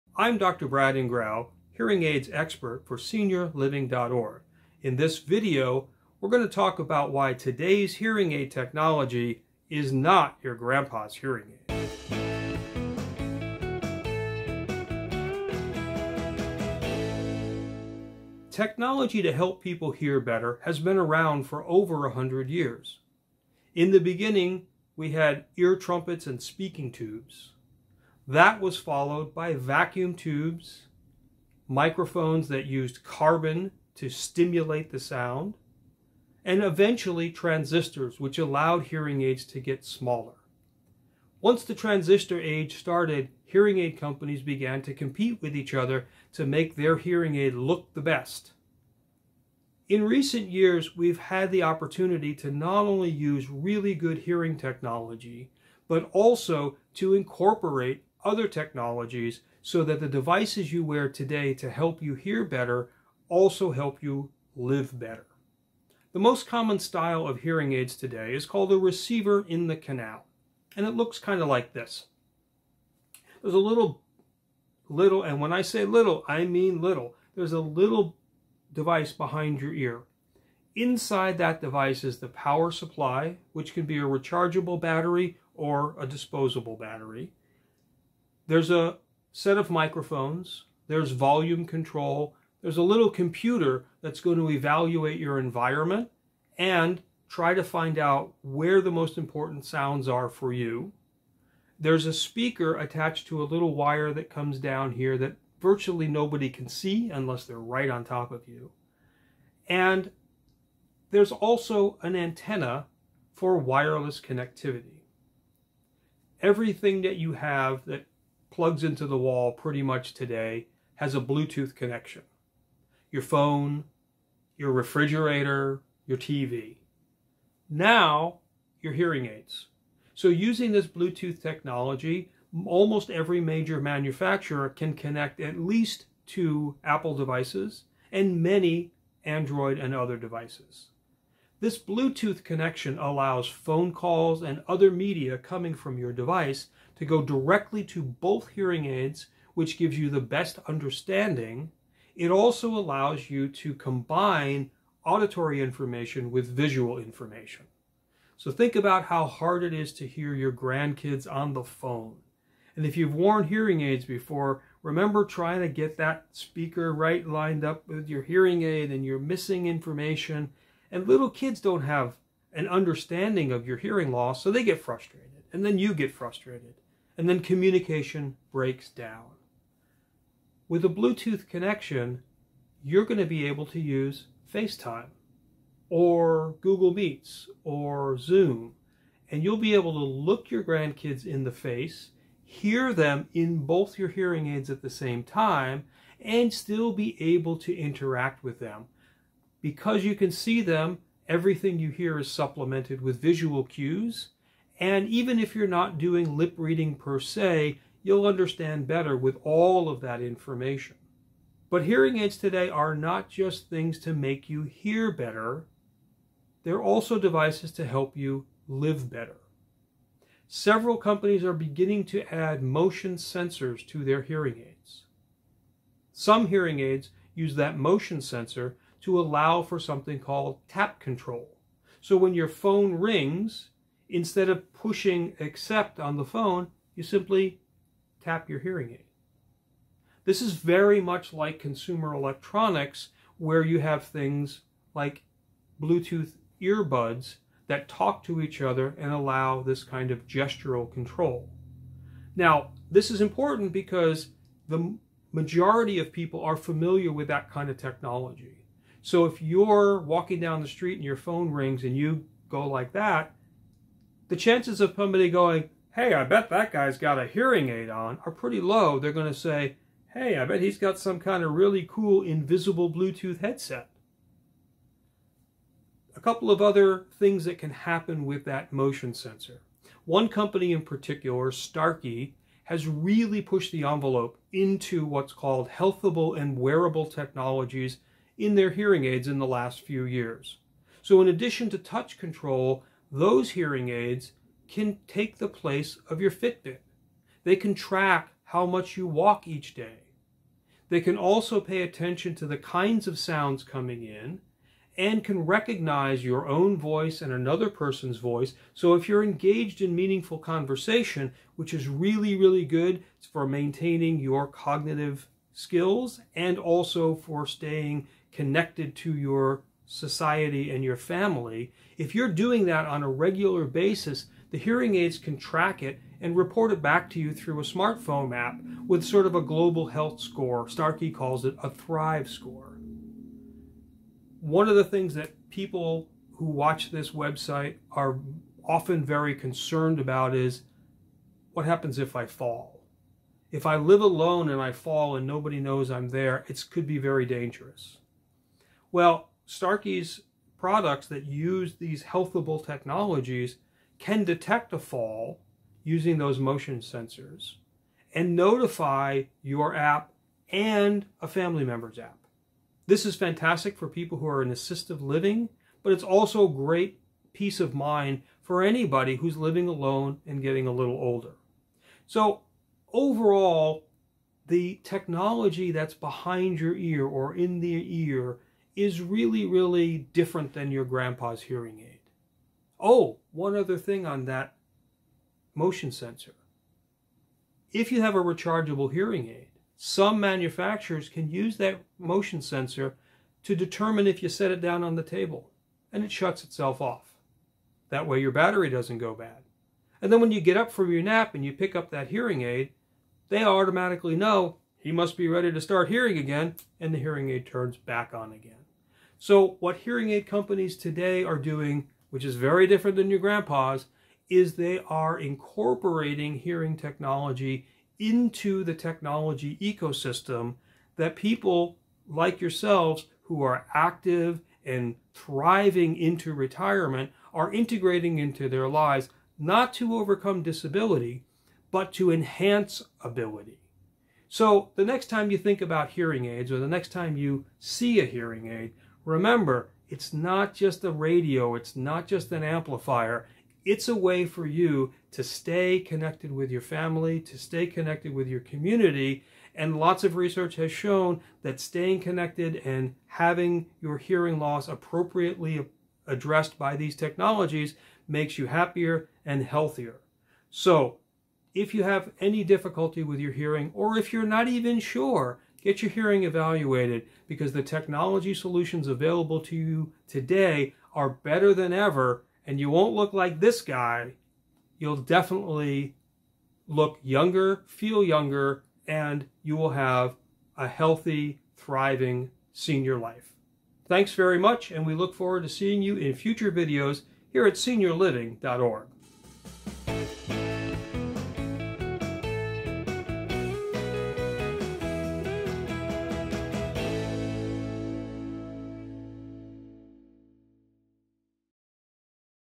Today's High Tech Hearing Aids are not your Grandpa's! with Brad Ingrao, AuD
Today's high tech hearing aids have come a long way from earlier models. In this video, Audiologist Brad Ingrao explains how today's high tech hearing aids can help you hear better than ever... and much, much more.

Time Stamps
0:00 Introducing Audiologist Brad Ingrao, AuD
0:19 The history of hearing aids, from ear trumpets to today’s high tech hearing aids
0:49 Today’s high tech hearing aids help you hear better and live better
1:50 Today’s high tech hearing aids employ wireless connectivity via Bluetooth technology
3:03 Bluetooth technology allows hearing aid wearers to use Zoom, FaceTime, and other virtual meeting apps
3:35 Motion sensors in today’s high tech hearing aids allow tap control
4:45 Starkey high tech hearing aids can track your steps like a Fitbit
5:40 Starkey high tech hearing aids can help detect falls and call for help
6:50 Today’s high tech hearing aids don’t just fight disability, but enhance ability

Like much modern technology, hearing aids have evolved from low-tech ear trumpets to high tech hearing aids that sport computers, motion sensors, rechargeable batteries, and more. Bluetooth technology allows people with hearing issues to connect both their ears to a phone call or FaceTime meeting. Some high tech hearing aids can be used as fitness trackers. Today's high tech hearing aids even can be used to detect falls, and then call for help, like a medical alert system.
Audiologist Brad Ingrao covers the history of hearing aids, from ear trumpets and speaking tubes to today's multifunctional high tech hearing aids that can be worn without being detected.

If you're interested in joining the hundreds of thousands of families who've found senior living communities on SeniorLiving.org, read the reviews on our

All early hearing aid photos courtesy of hearingaidmuseum.com